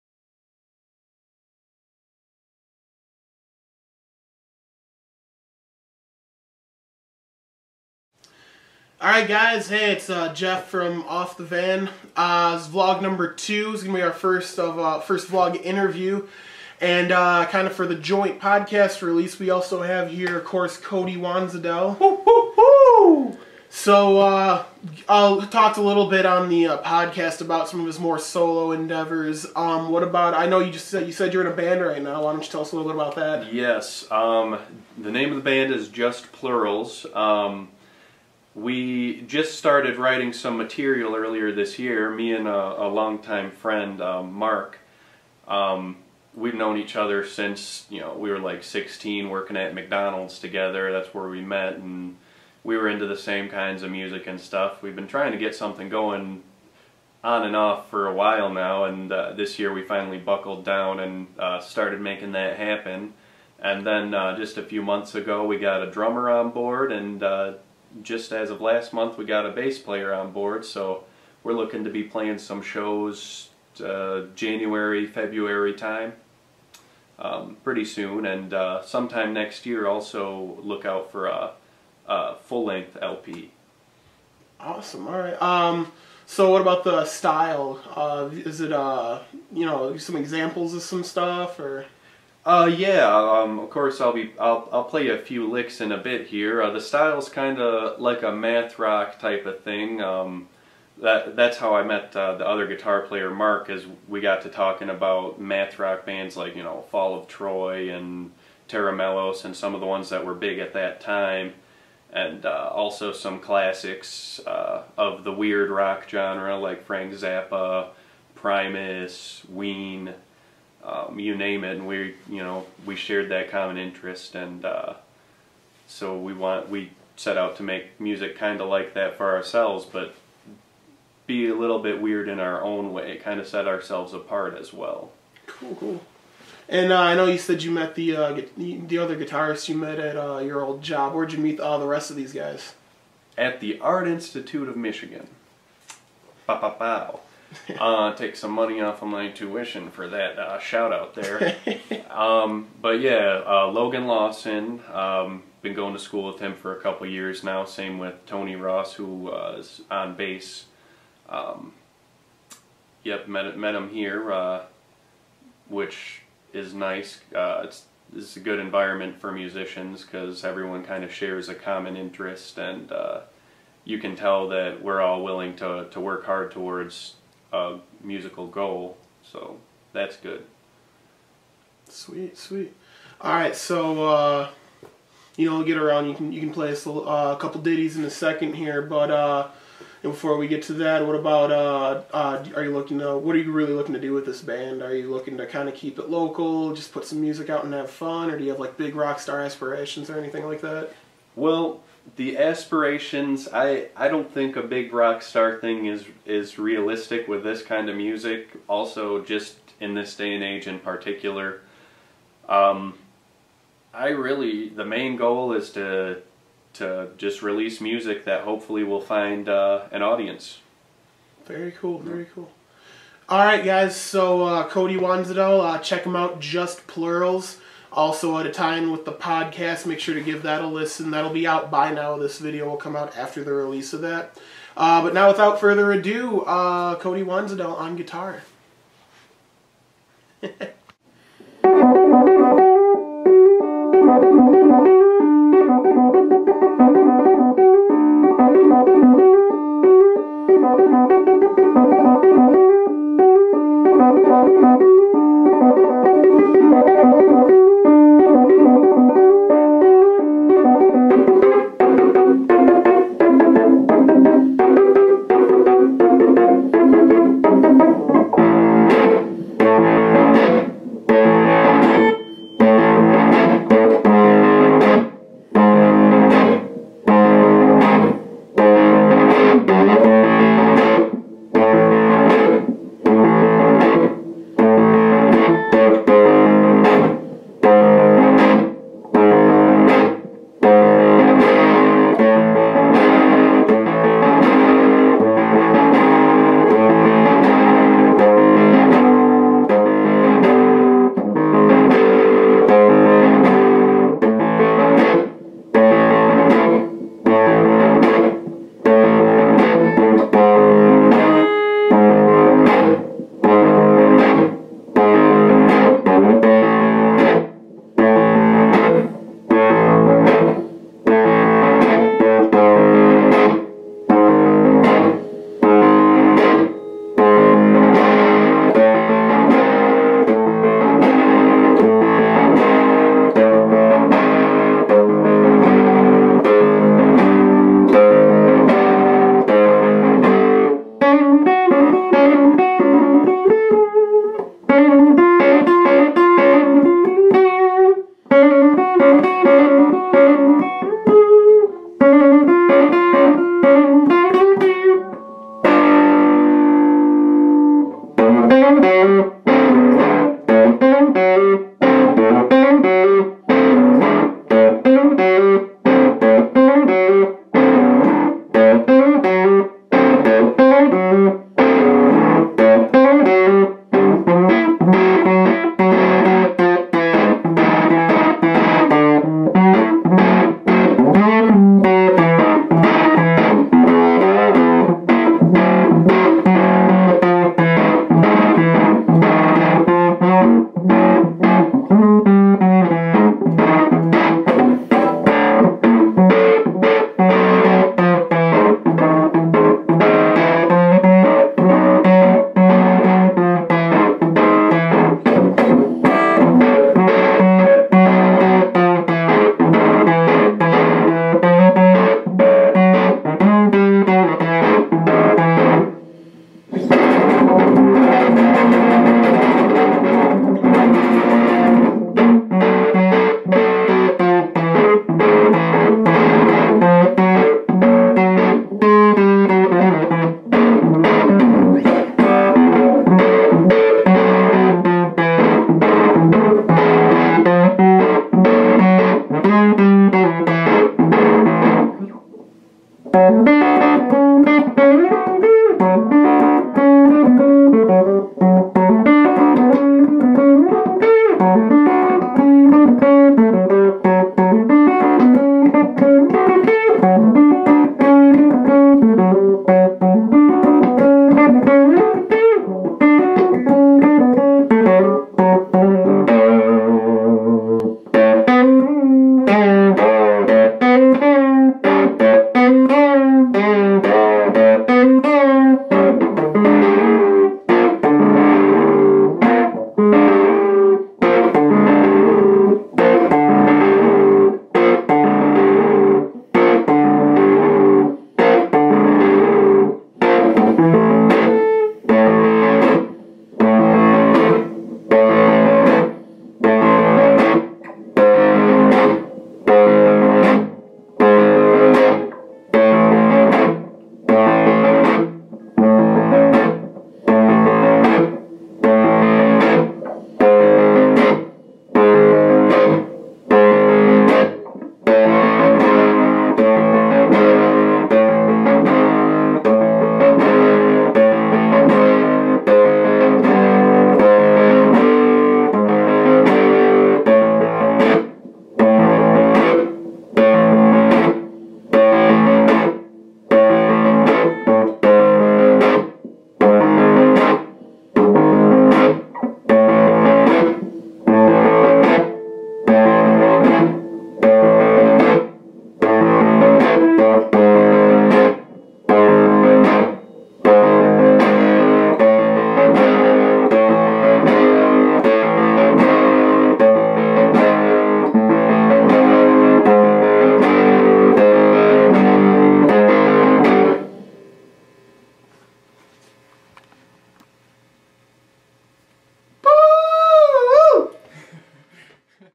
Off The Van Vlog 02 - Cody Wansedel
We chat with Cody Wansedel and he show us some sweet licks from his new band Just Plurals